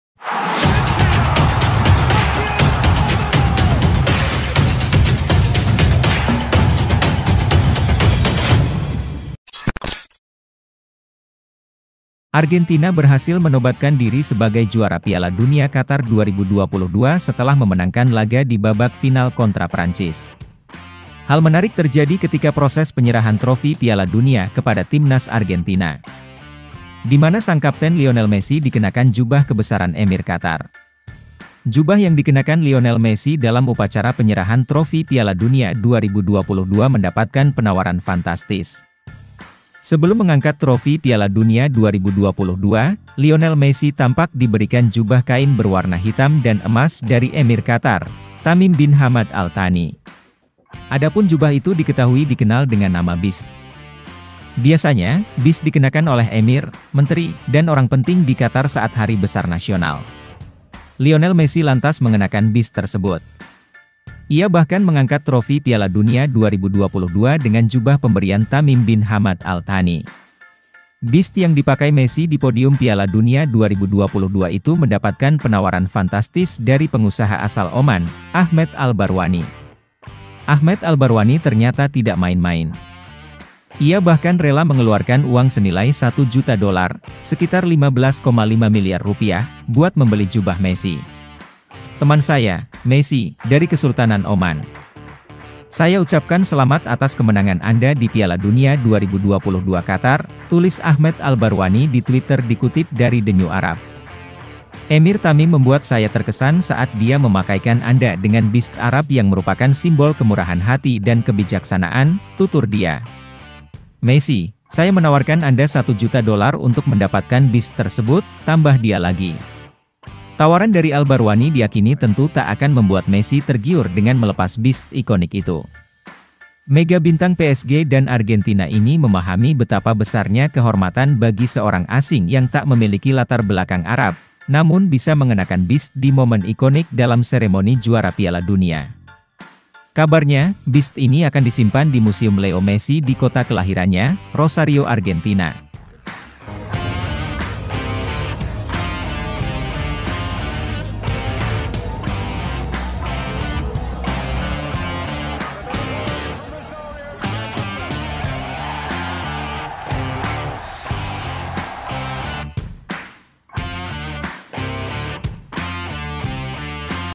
Jubah Bisht Messi Ditawar Jutaan Dolar Oleh Kesultanan Oman
Jubah Bisht Messi yang dipakainya saat penyerahan tropi juara Piala Dunia Qatar 2022 ditawar jutaan dolar oleh Kesultanan Oman. Ialah anggota Parlemen Oman , Ahmed Al Barwani. Jubah Bisht yang diberikan secara langsung oleh Emir Qatar ke Lionel Messi tersebut tak tanggung-tanggung ia tawar US$ 1 Juta atau sekitar Rp15,5 Miliar untuk mendapatkan jubah Bisht Messi tersebut.